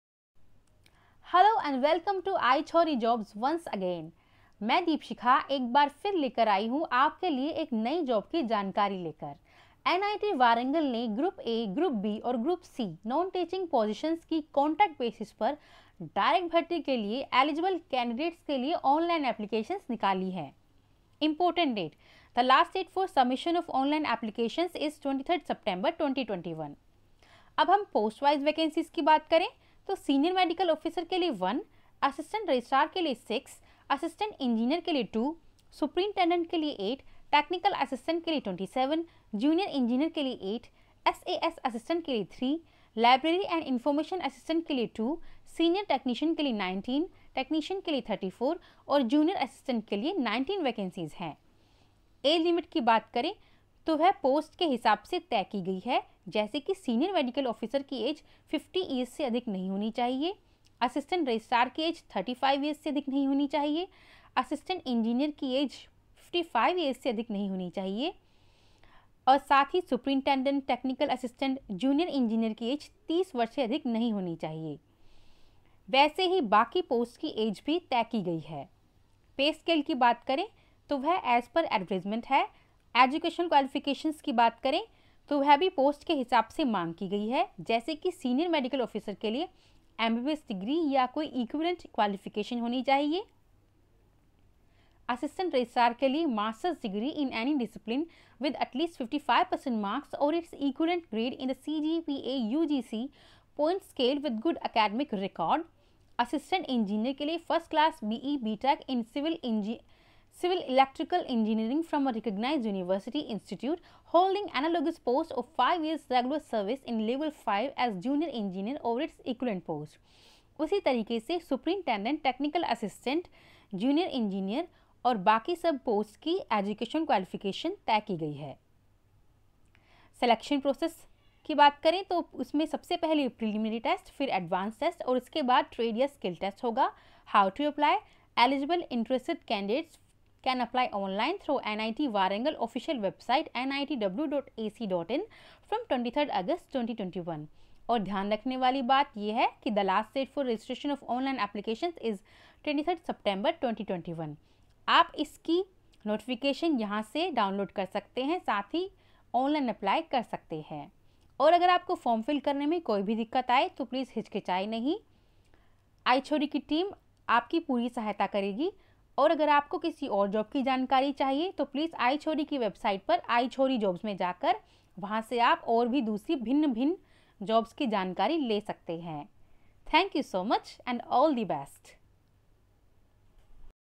NIT Warangal Recruitment 2021 ,129 Vacancies
NIT Warangal Recruitment 2021 invites online applications from eligible candidates for direct recruitment of the following Group A, Group B and Group C Non Teaching positions on regular basis/ direct recruitment on contract basis/ deputation basis.

Important Date:

The last date for submission of online applications is 23rd September 2021.

NIT Warangal Recruitment 2021

Post Name

No of Vacancies

Senior Medical Officer

01

Assistant Registrar

06

Assistant Engineer

02

Superintendent

08

Technical Assistant

27

Junior Engineer

08

SAS Assistant

03

Library and Information Assistant

02

Senior Technician

19

Technician

34

Junior Assistant

19

Age Limit:

Senior Medical Officer: Not exceeding 50 years

Assistant Registrar: Not exceeding 35 years

Assistant Engineer: Not exceeding 55 years

Superintendent: Not exceeding 30 years

Technical Assistant: Not exceeding 30 years

Junior Engineer: Not exceeding 30 years

 SAS Assistant: Not exceeding 30 years

Library and Information Assistant: Not exceeding 30 years

Senior Technician: Not exceeding 33 years

Technician / Junior Assistant: Not exceeding 27 years

Pay Scale: As per Advertisement.

Educational Qualifications:

Senior Medical Officer: MBBS Degree or equivalent qualification.

Assistant Registrar: Master’s degree in any discipline with at least 55% marks or its equivalent Grade in the CGPA/ UGC point scale with good academic record.

Assistant Engineer: First Class B.E /B.Tech in Civil/Electrical Engineering from a recognized University / Institute. Holding analogous post or 5 years’ regular service in Level 5 as Junior Engineer or its equivalent post.

Superintendent: Master’s degree in any discipline with at least 50% marks or its equivalent Grade. Knowledge of Computer applications, viz. word processing, spread sheet, etc.

Technical Assistant: First Class or equivalent Grade in B.E./B. Tech in Computer Science & Engineering / Instrumentation Engineering / Electronics and Communication Engineering / Electrical & Electronics Engineering /Mechanical Engineering / Civil Engineering / Metallurgical & Material Engineering / Chemical Engineering / Biotechnology.

Junior Engineer: First Class B.E./ B. Tech in Civil/ Electrical Engineering.

SAS Assistant: First Class Bachelor’s Degree in Physical Education.

Library and Information Assistant: First Class Bachelor’s Degree in Science/Arts/Commerce from a recognized University / Institute and Bachelor’s Degree in Library and Information Science.

Senior Technician: Diploma in Engineering in Civil / Electrical / Mechanical / Computer Science / Electronics & Communication / Information Technology of three year’s duration in relevant field.

Technician / Junior Assistant: Diploma in Engineering in Civil / Electrical / Mechanical / Computer Science / Electronics & Communication / Information Technology of three year’s duration in relevant field from a Government recognized Polytechnic / Institute. (OR) Senior Secondary (10+2) from a government recognized board with at least 50% marks and ITI course of one year or higher duration in Civil / Electrical / Mechanical / Electronics / DMLT / Photography trade/Automobile Engg.

Selection Process:

Preliminary Test

Advanced Test

Trade / Skill Test

How to Apply: Eligible Interested candidates for apply online through NIT Warangal official website (nitw.ac.in) from 23rd August 2021. The last date for registration of online applications is 23/09/2021.